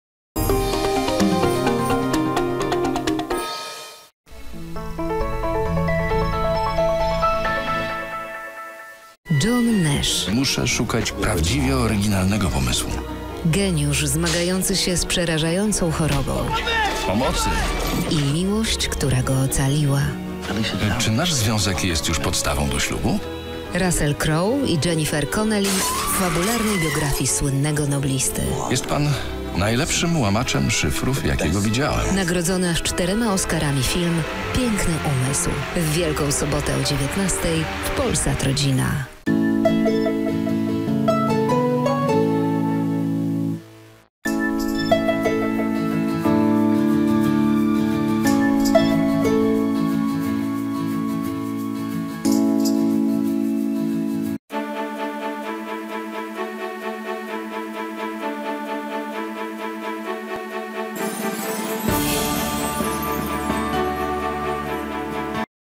Polsat Rodzina - Restart emisji (06.04.2020)
Źródło: IPTV Cyfrowy Polsat GO - film nagrany z użyciem MIUI Recorder
Prawa autorskie: Telewizja Polsat
Wszystkie kanały Polsatu wtedy zmieniają oprawę a Polsat Rodzina Games News nie zmienia